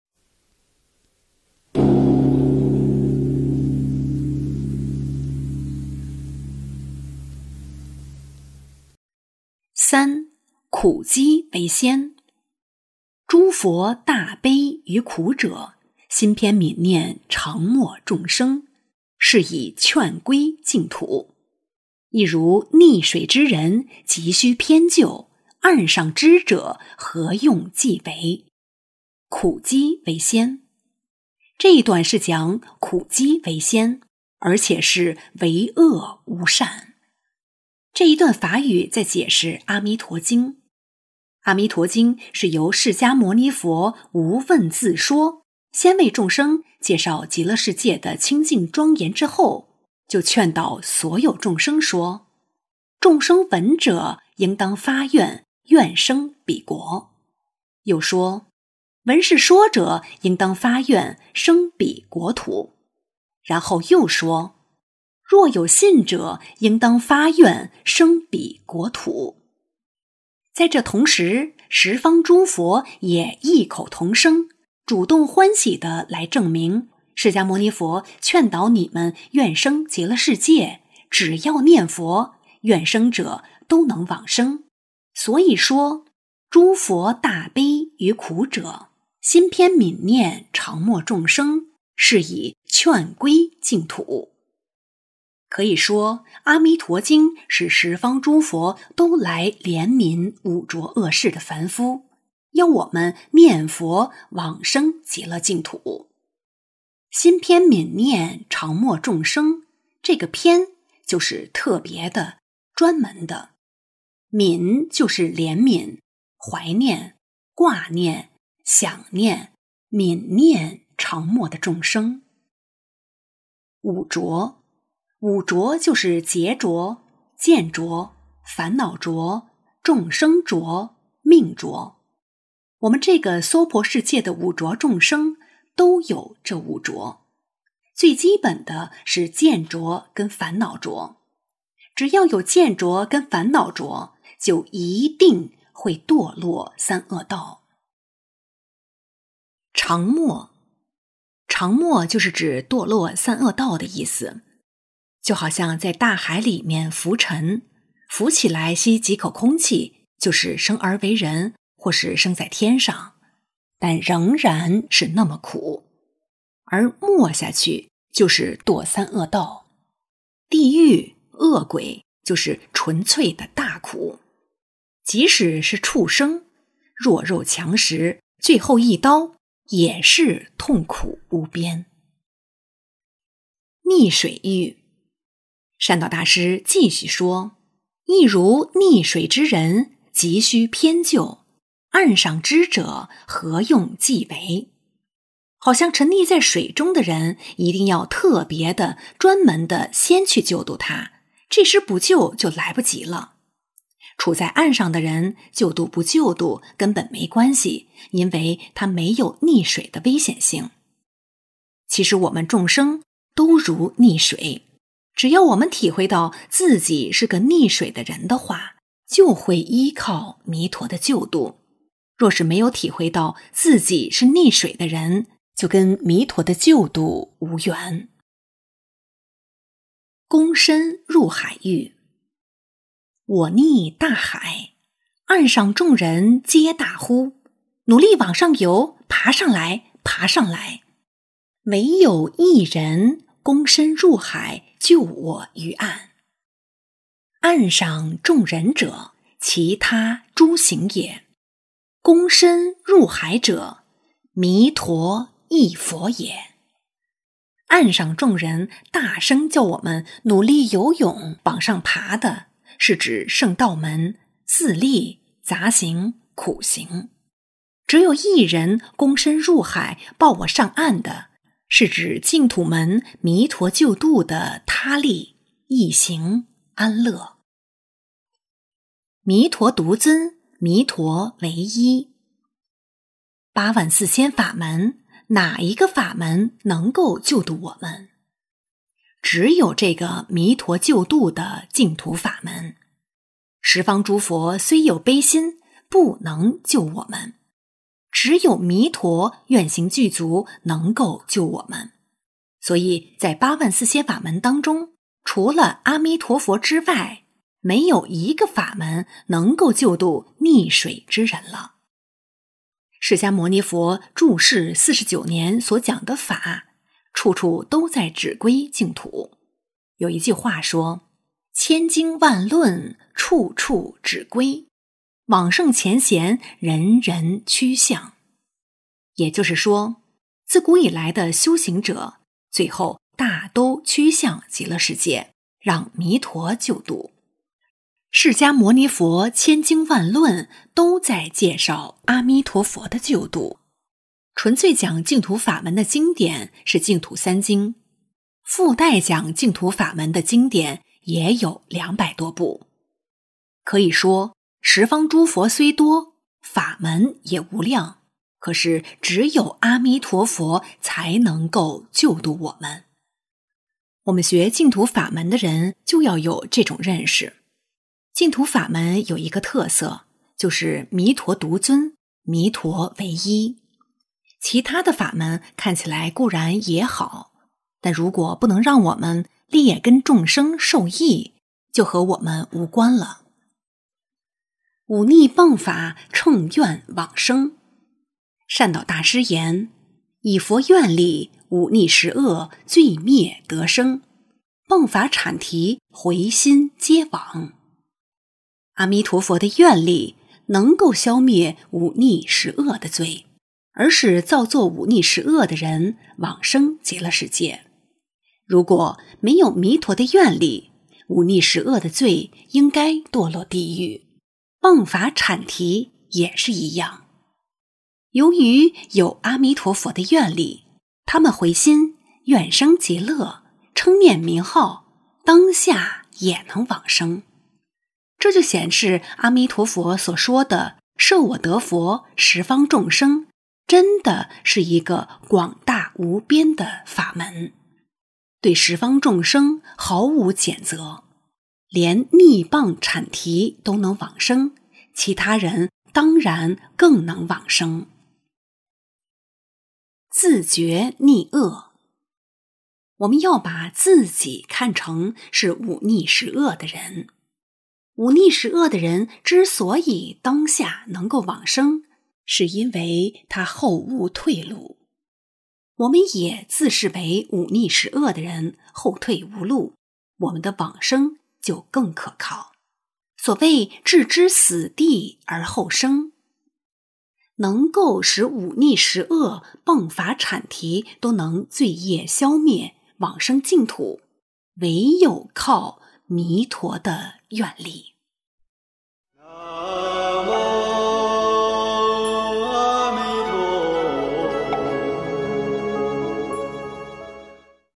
躬身入海者，彌陀一佛也｜30第十八願講話｜慧淨法師文章
躬身入海者，彌陀一佛也｜30第十八願講話｜慧淨法師文

淨土宗澳洲 Pure Land Buddhism Australia

净土宗官方網站
The official website of the Pure Land Buddhism：

三、苦機為先

諸佛大悲於苦者，心偏愍念常沒眾生，是以勸歸淨土。 亦如溺水之人，急須偏救;岸上之者，何用濟為。 -- 苦機為先

這一段是講「苦機為先」，而且是「唯惡無善」。

這一段法語在解釋《阿彌陀經》。《阿彌陀經》是由釋迦牟尼佛無問自說，先為眾生介紹極樂世界的清淨莊嚴之後，就勸導所有眾生說:
    眾生聞者，應當發願，願生彼國。
又說:
    聞是說者，應當發願，生彼國土。
然後又說:
    若有信者，應當發願，生彼國土。

在這同時，十方諸佛也異口同聲，主動歡喜地來證明:「釋迦牟尼佛勸導你們願生極樂世界，只要念佛，願生者都能往生!」所以說「諸佛大悲於苦者，心偏愍念常沒眾生，是以勸歸淨土」。
可以說，《阿彌陀經》是十方諸佛都來憐憫五濁惡世的凡夫，要我們念佛往生極樂淨土。

「心偏愍念常沒眾生」:這個「偏」就是特別的、專門的;「愍」就是憐憫、懷念、掛念、想念，愍念常沒的眾生。

五濁

 「五濁」:就是劫濁、見濁、煩惱濁、眾生濁、命濁。我們這個娑婆世界的五濁眾生，都有這五濁，最基本的是見濁跟煩惱濁，只要有見濁跟煩惱濁，就一定會墮落三惡道。

常沒

「常沒」: 就是指墮落三惡道的意思。就好像在大海裏面浮沉，浮起來吸幾口空氣-- 就是生而為人，或是生在天上，但仍然是那麼苦;而沒下去 -- 就是墮三惡道，地獄、餓鬼，就是純粹的大苦，即使是畜生，弱肉強食， 最後一刀也是痛苦無邊。

溺水喻

 善導大師繼續說:
 「亦如溺水之人，急須偏救;岸上之者，何用濟為」: 好像沉溺在水中的人，一定要特別地、專門地先去救度他，這時不救就來不及了。處在岸上的人，救度不救度，根本沒關係，因為他沒有溺水的危險性。

其實我們眾生都如溺水，只要我們體會到自己是個溺水的人的話，就會 依靠彌陀的救度;若是沒有體會到自己是溺水的人，就跟彌陀的救度無緣。

躬身入海喻

我溺大海，岸上眾人皆大呼:「努力往上游!爬上來!爬上來!」
唯有一人，躬身入海，救我於岸。 岸上眾人者，其他諸行也;躬身入海者，彌陀一佛也。

岸上眾人大聲叫我們努力游泳、往上爬的，是指聖道門，自力、雜行、苦行。
只有一人躬身入海、抱我上岸的，是指淨土門彌陀救度的他力、易行、安樂。

彌陀獨尊、彌陀唯一

八萬四千法門，哪一個法門能救度我們?只有這個彌陀救度的淨土法門。
十方諸佛，雖有悲心，不能救我們。只有彌陀，願行具足，能夠救我們。 所以，在八萬四千法門當中，除了阿彌陀佛之外，沒有一個法門能夠救度 溺水之人了。釋迦牟尼佛住世四十九年所講的法，處處都在指歸淨土，有 一句話說:「千經萬論，處處指歸;往聖前賢，人人趨向」。也就是說， 自古以來的修行者，最後大都趨向極樂世界，讓彌陀救度。釋迦牟尼佛千 經萬論都在介紹阿彌陀佛的救度-- 純粹講淨土法門的經典，是淨土三經， 附帶講淨土法門的經典，也有兩百多部。可以說，十方諸佛雖多，法門也無量，可是只有阿彌陀佛才能夠救度我們。

我們學淨土法門的人，就要有這種認識。淨土法門有一個特色，就是「彌陀獨尊、彌陀唯一」，其他的法門看起來固然也好，但如果不能讓我們劣 根眾生受益，就和我們無關了。

五逆謗法，乘願往生
善導大師言: 以佛願力:五逆十惡，罪滅得生;謗法闡提，迴心皆往。
阿彌陀佛的願力，能夠消滅五逆十惡的罪，而使造作五逆十惡的人往生極樂世界。如果沒有彌陀的願力，五逆十惡的罪應該墮落地獄，謗法闡提也是一樣; 由於有彌陀的願力，他們迴心，願生極樂，稱念名號，當下也能往生。

這就顯示阿彌陀佛所說的「設我得佛，十方眾生」真的是一個廣大無邊的法門，對十方眾生毫無揀擇，連逆謗闡提都能往生，其他人當然更能往生。

自覺逆惡
我們要把自己看成是五逆十惡的人。五逆十惡的人之所以當下能夠往生， 是因為他後無退路;我們也自視為五逆十惡的人，後退無路，我們的往生 就更可靠，所謂「置之死地而後生」。能夠使五逆十惡、謗法闡提，都能 罪業消滅、往生淨土，唯有靠彌陀的願力。